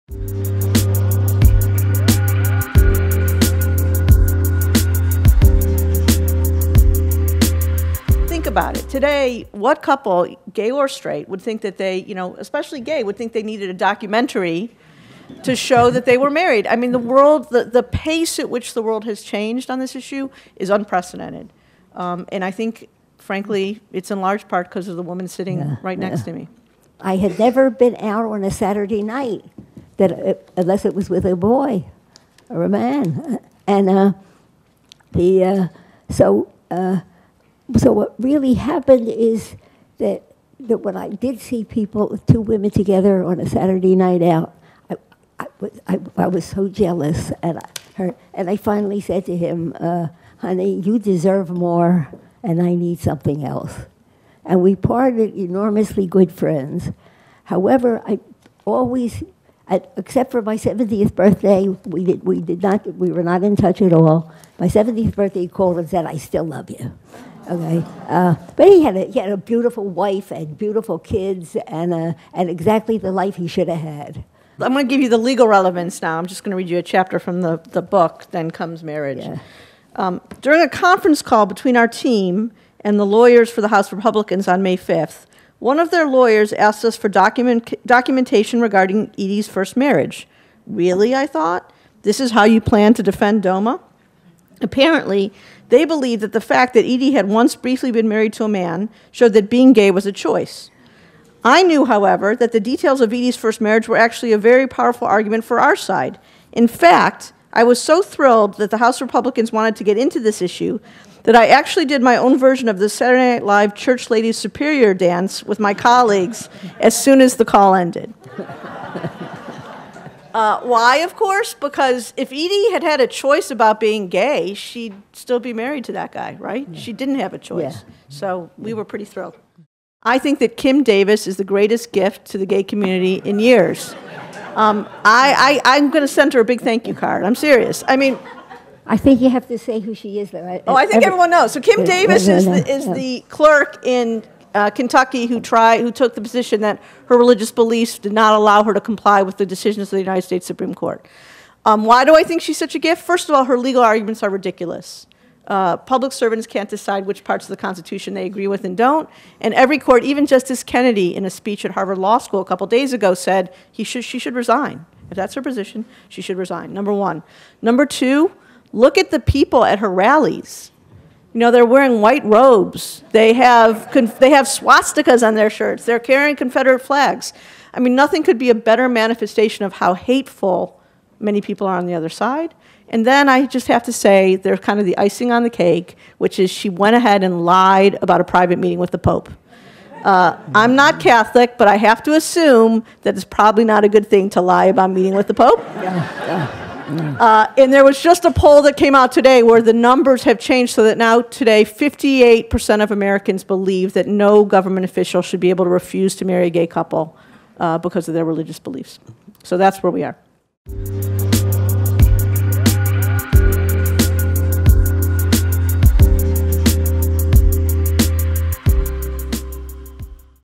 Edith Windsor and Roberta Kaplan: From Overturning DOMA to Kim Davis | The New School
Excerpt from "Edith Windsor: An Unlikely Activist" Presented by the Institute for Retired Professionals (IRP at The New School

From private citizen to documentary film star to Supreme Court petitioner who overturned the Defense of Marriage Act (DOMA), Edith (Edie) Windsor became a gay rights and feminist icon in her ninth decade. In a conversation between Edie and her attorney, Roberta Kaplan, they trace the odyssey through the court system resulting in United States v. Windsor - the 2013 landmark decision that struck down DOMA.